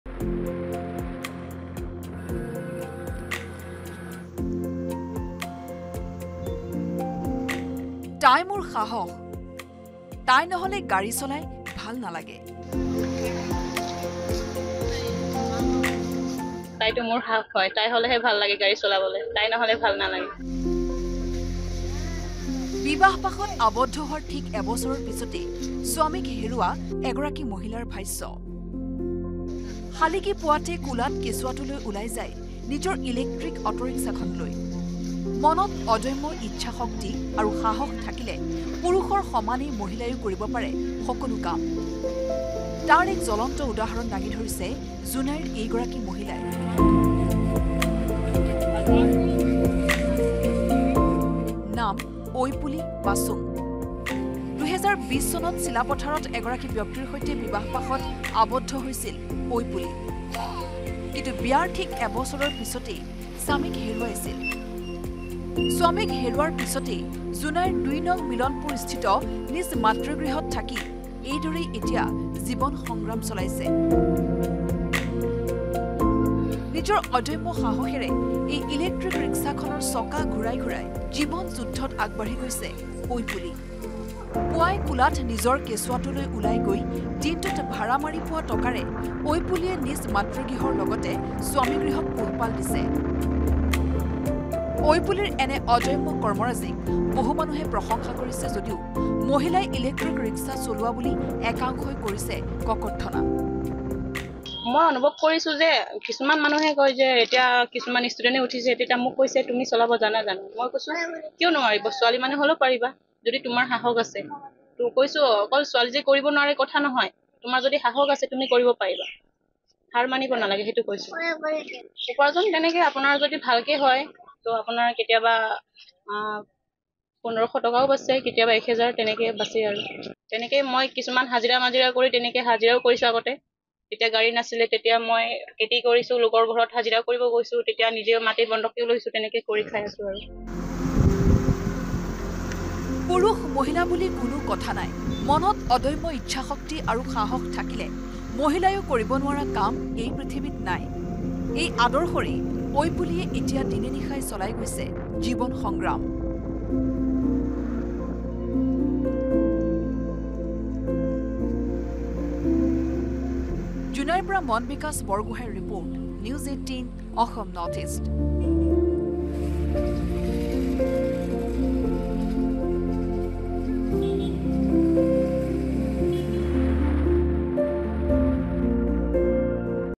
Life Struggle of a lady : জোনাইৰ ঐপুলি পাচুঙৰ জীৱন সংগ্ৰামৰ কাহিনী | N18V
Life Struggle of a lady | জোনাইৰ ঐপুলি পাচুঙৰ জীৱন সংগ্ৰামৰ কাহিনী। শালিকী পুৱাতে কোলাত কেঁচুৱা লৈ ওলাই যায় ইলেক্টিক -অটোৰিক্সা চলাবলৈ। ৰদ বৰষুণ একাকাৰ কৰি কষ্টোপাৰ্জিত ধনৰে পোহপাল দিয়ে দুখনকৈ ঘৰ। ঐপুলিৰ সংগ্ৰাম দেখিলে আপোনাৰো দুচকুৰে নিগৰিব অশ্ৰু ।